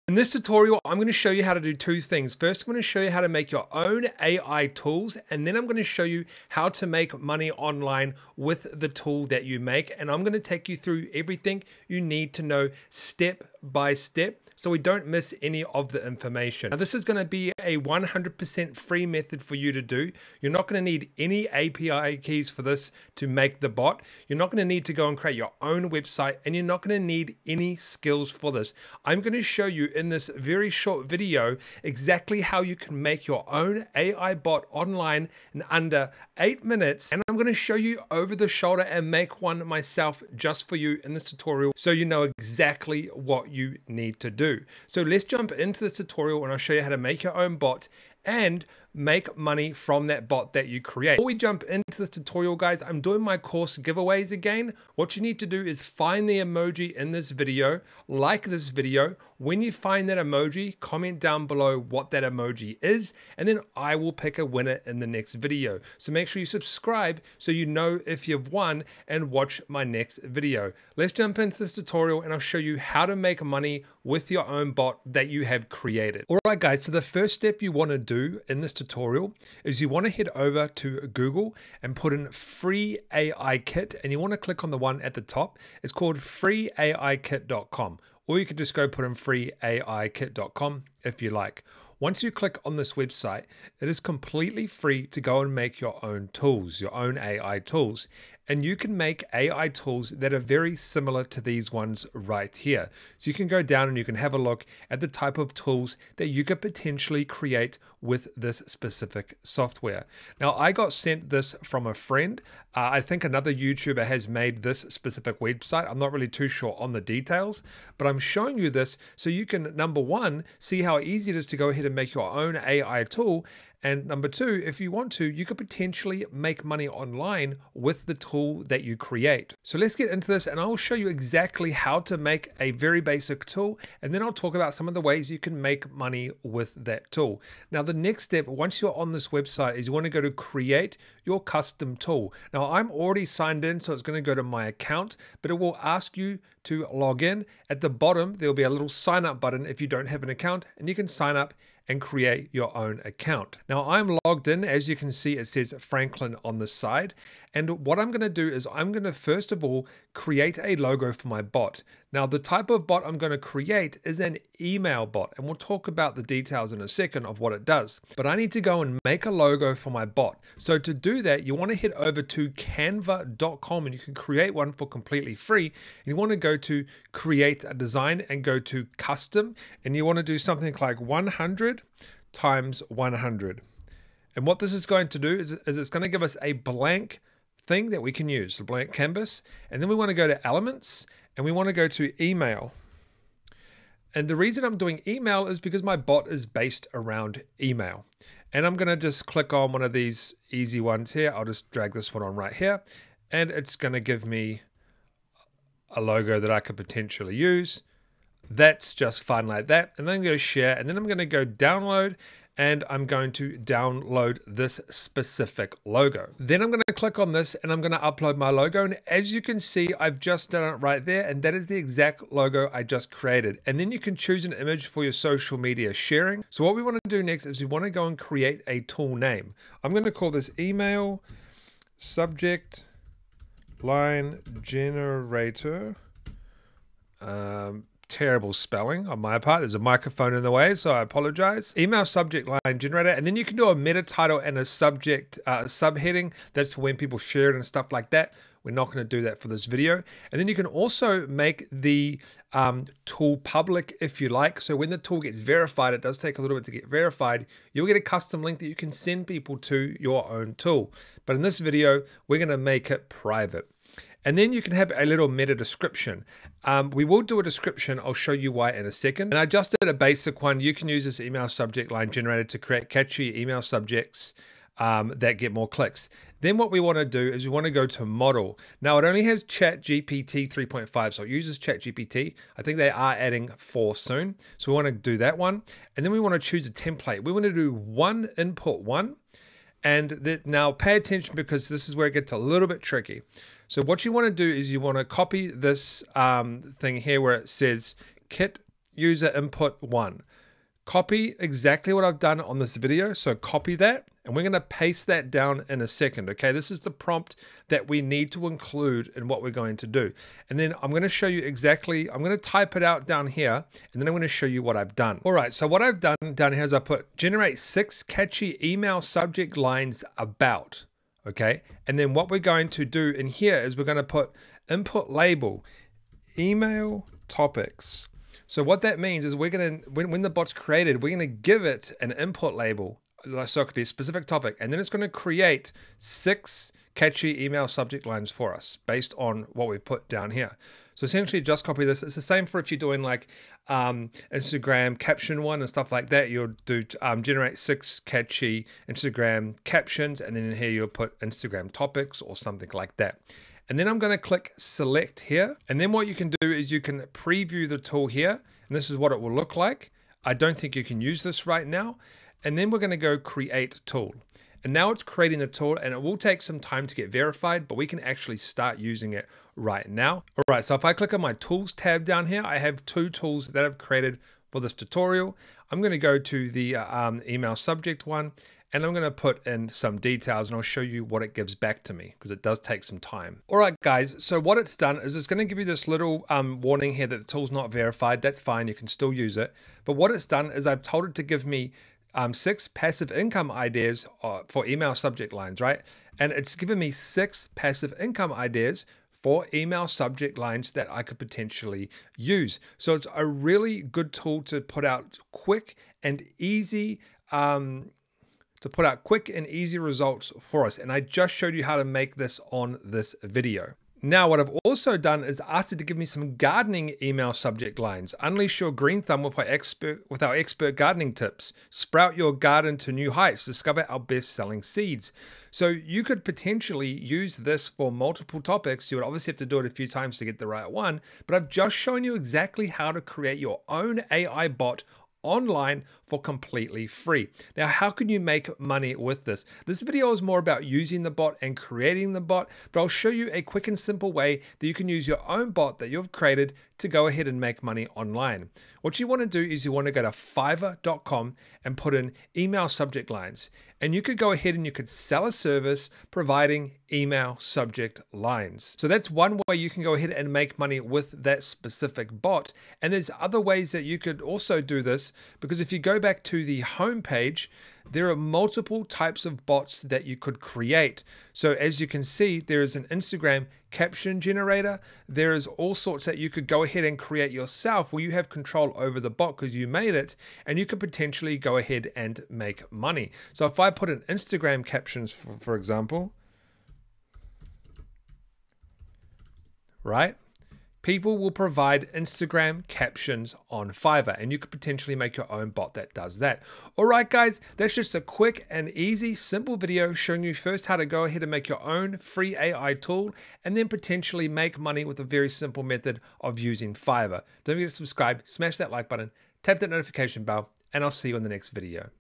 How To Create A.I Bots FREE Without Any Skills Then Make Money Online! (In 7 Minutes)
I show you how to make A.I bots then make money online. This free tutorial will show you how to make A.I tools using a free online website then I will show you some ways that you can use that tool to make money online.

Head over to FreeAiKit.com and start to use the free A.I bot making software. You can create any type of bot you can think of using ChatGPT and prompts. If you want to create your own easy A.I bot then create something like a email subject line generator like I do in the tutorial.

Creating a bot using A.I is easy to do and you can them make money online using that bot you created. You can use the toll you create to start a business online making money with A.I.